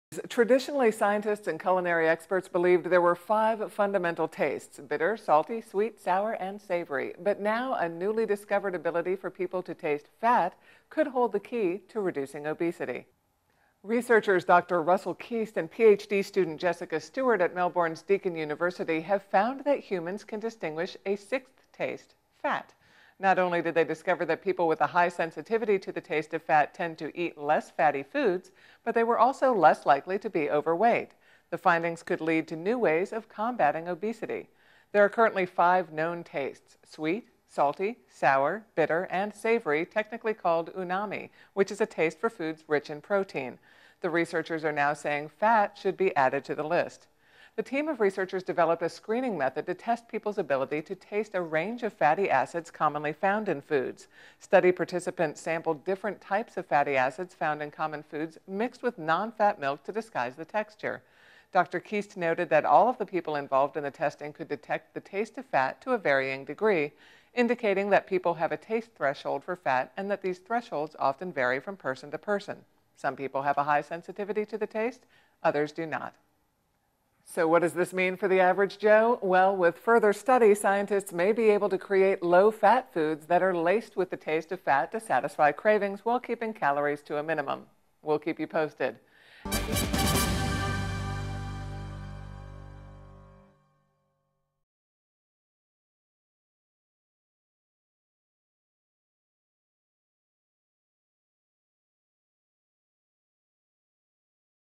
Fat Found To Be Sixth Taste
In addition to the five fundamental tastes, humans can also taste fat, according to a new study. The research could lead to innovative weight loss products..